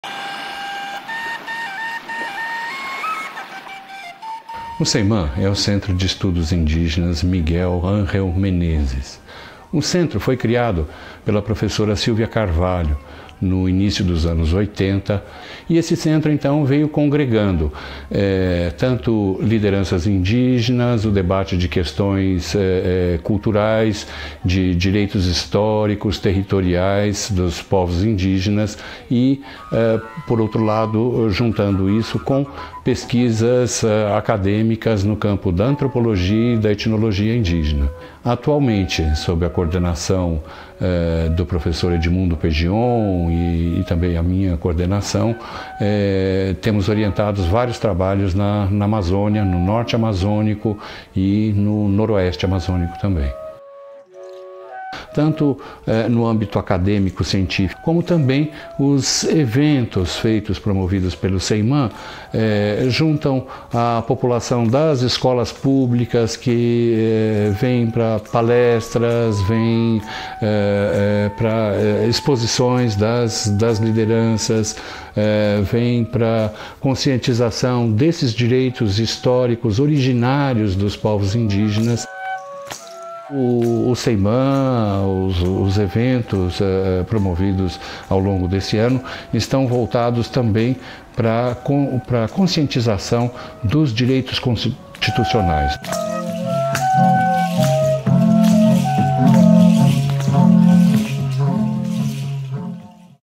Educando para a Diversidade | Ceimam é referência para os estudos dos povos indígenas.
Ceimam é referência para os estudos dos povos indígenas.
Conheça um centro que é referência para os estudos dos povos indígenas na Unesp de Araraquara.